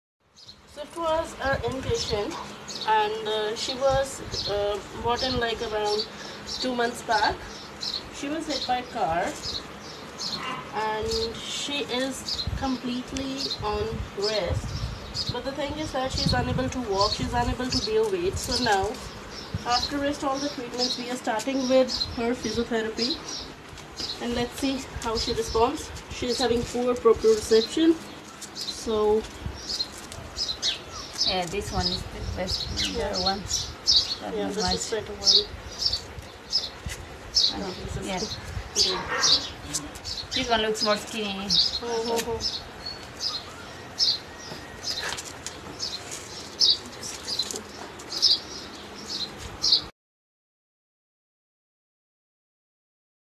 DAR Diaries - Swift (part 2)
Do you remember Swift? The 5y/o who was hit by a car in Gharoh and had lost all sense of her body? He's fully recovered!

Thank you for your continued support! This is all made possible because of your help.

Any amount truly matters and is much appreciated!